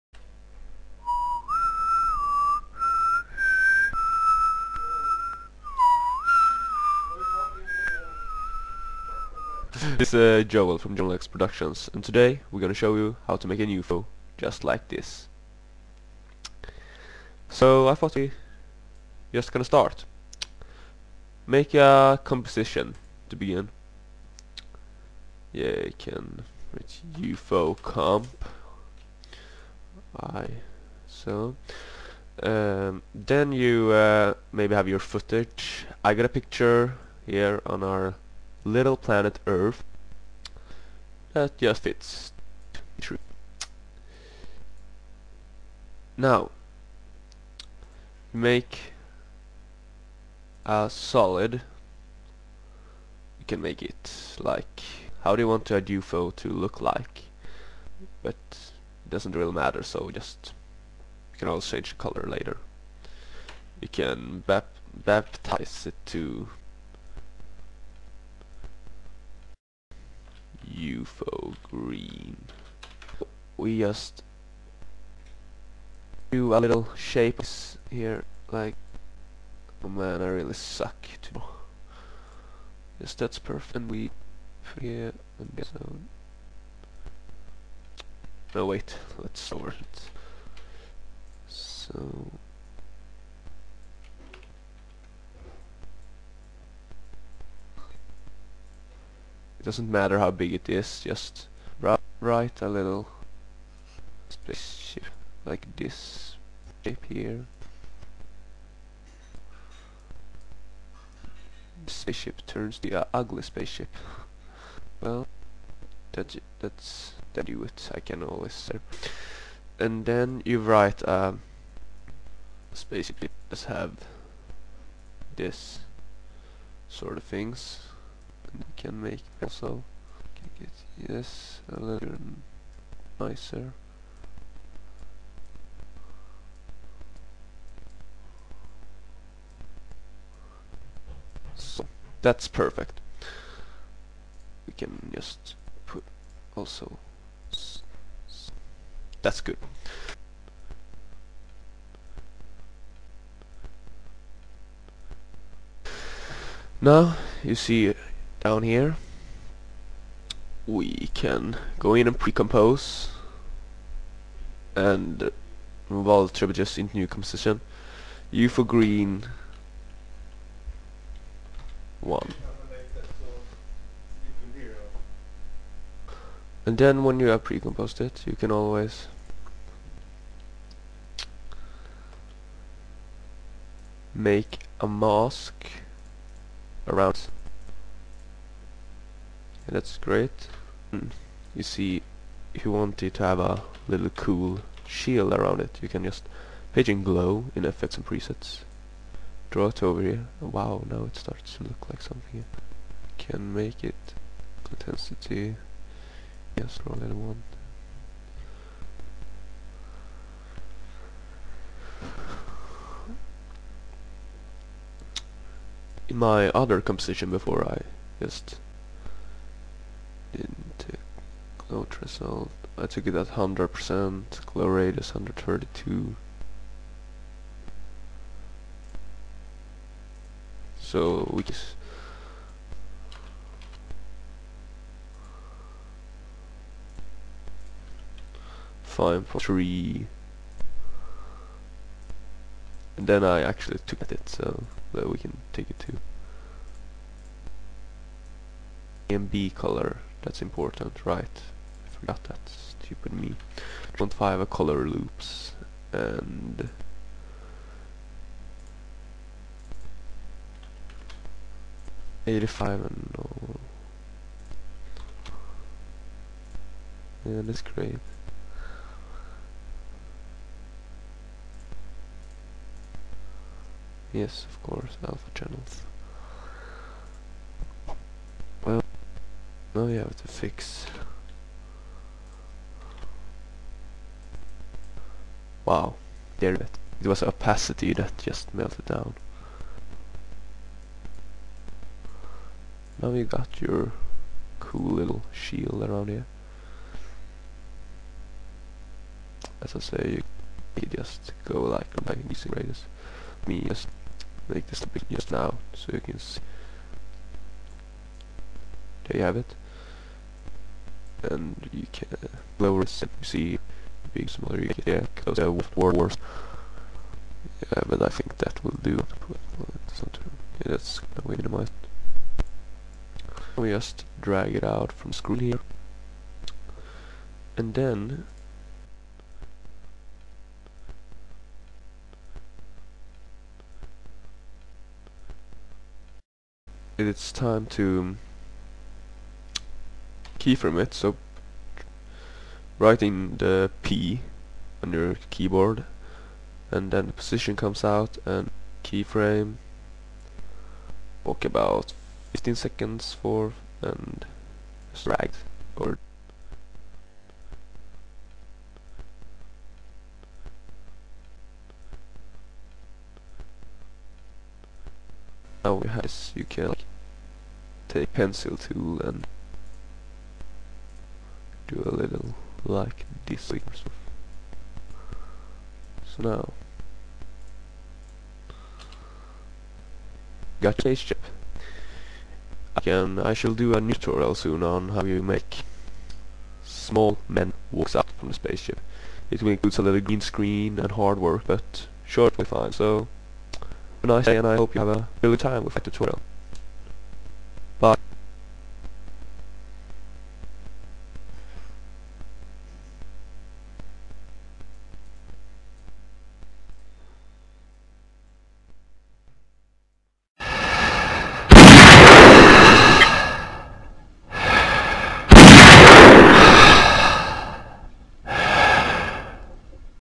How to make a Spaceship/UFO in After Effects
This is our first tutorial ever! Well how i come up with the idea "how to make a spaceship/UFO in after effects" was because i noticed that NO ONE on youtube had done it! I thought it was terrible so i did one myself and hope some people will be needing it in the future. Thank you and have good day. =)

// Joel And Alex Productions

PS: In the end you might not hear it because the sound was weird but i say :
Soon i will do a new tutorial on how to make small green men walk out from the spaceship.
It will include greenscreen and hard work. Hope you liked this tutorial. Bye! =)